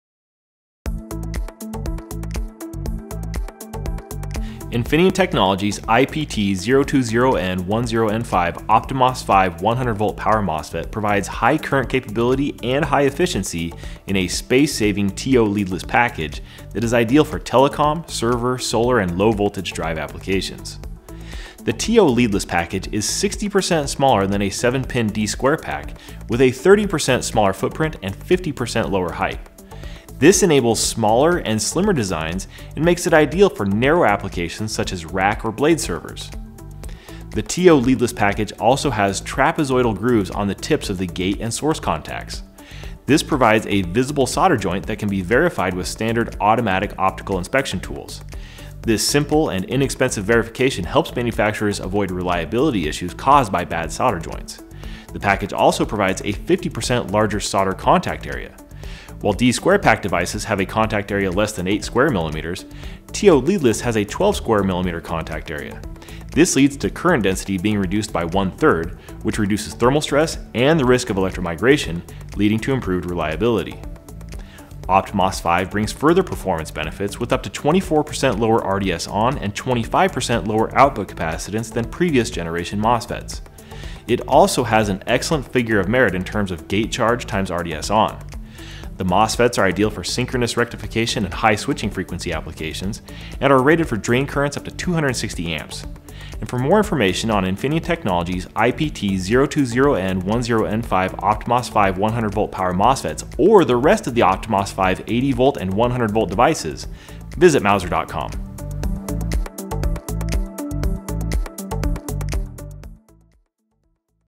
Infineon Technologies OptiMOS™ 5 Power MOSFETs | Featured Product Spotlight
Infineon Technologies’ IPT020N10N5 OptiMOS 5 100V power MOSFET provides high current capability and high efficiency in a space-saving TO-Leadless package that is ideal for telecom, server, solar, and low voltage drive applications.